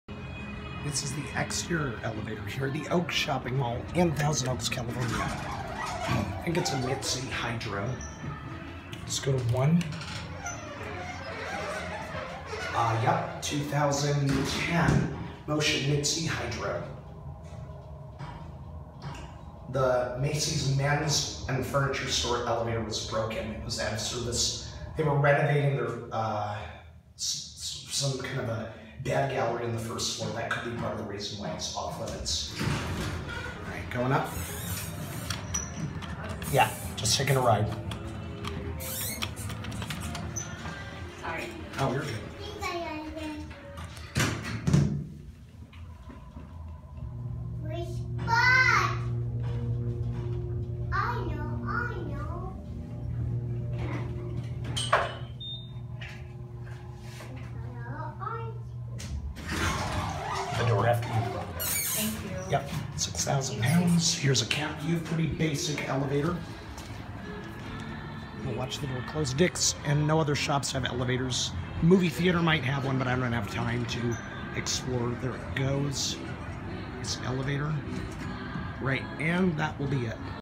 Mitsubishi/MCE Hydraulic External Elevator @ The Oaks, Thousand Oaks, CA
Pretty nice elevator. Installed in 2010.